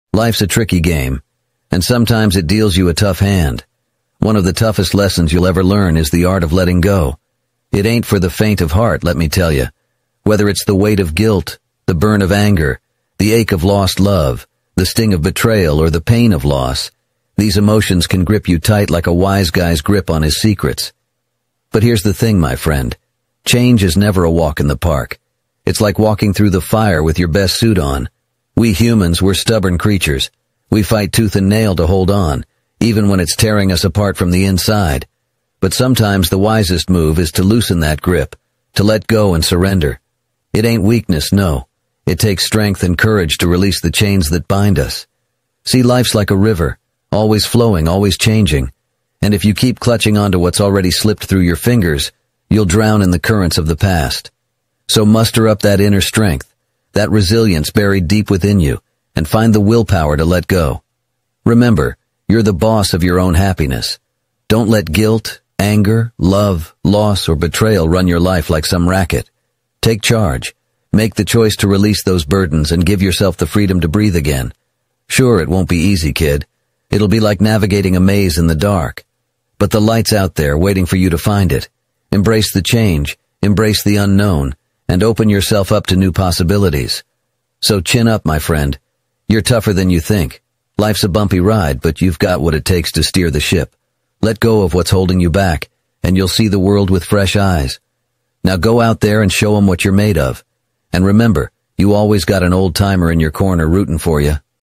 The Hardest Lesson in Life: Letting Go
In this video, we dive deep into one of life's toughest lessons: letting go. Whether it's guilt, anger, love, loss, or betrayal, we all face the challenge of embracing change. Join me on this transformative journey as we explore the power of releasing what no longer serves us.

In this inspiring and thought-provoking video, we discuss practical strategies for navigating the path of letting go. Discover how to find inner strength, overcome obstacles, and open yourself up to new beginnings. It's time to break free from the chains that hold you back and step into a brighter future.

Watch as we explore the emotional landscapes and provide actionable insights to help you on your own letting go journey. This video is a reminder that you have the power to choose your own happiness and create a life filled with purpose and joy.

Thank you for joining me on this empowering exploration. Together, we can conquer the challenges and live our best lives.